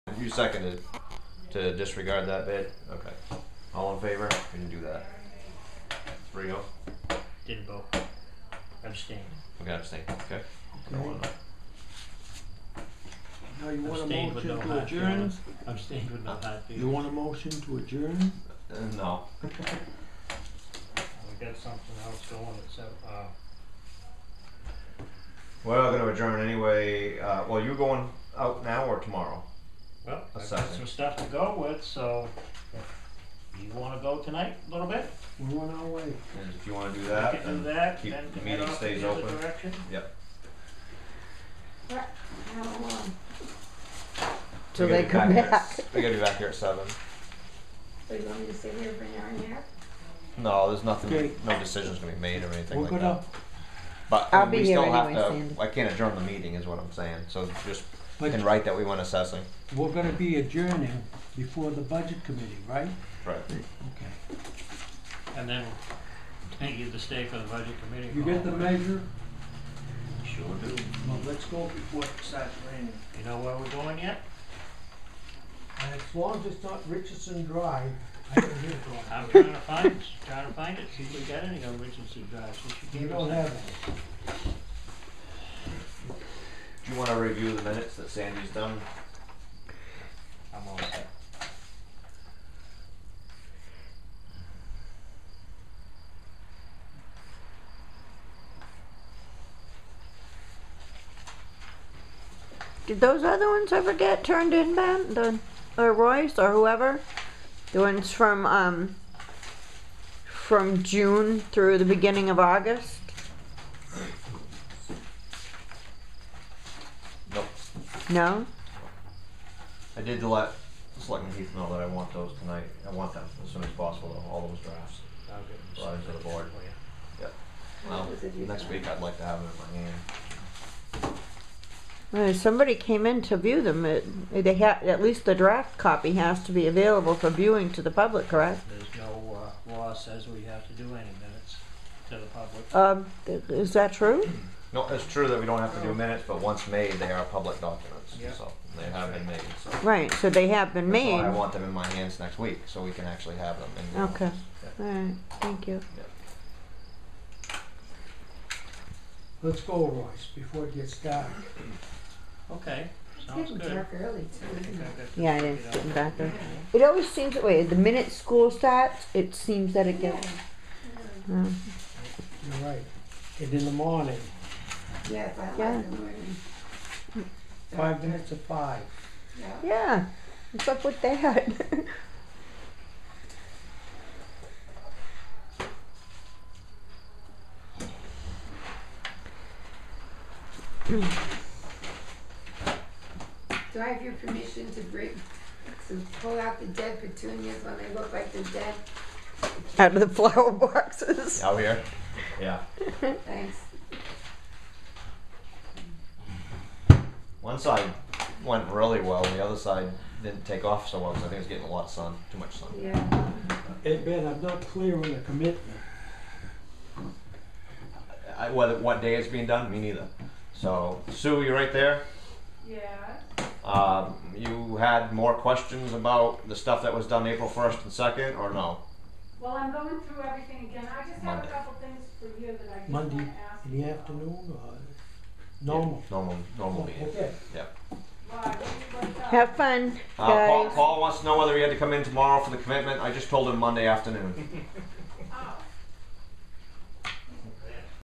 Lebanon Maine Selectmen's Meeting, 9/10/15, Part 7 (FINAL)
Board leaves the Town Office to complete assessment duties. Discussion on missing Board of Selectmen Meeting minutes from June-August, 2015. Board ready to complete 2015-16 Tax Commitment on Monday, 9/14/15.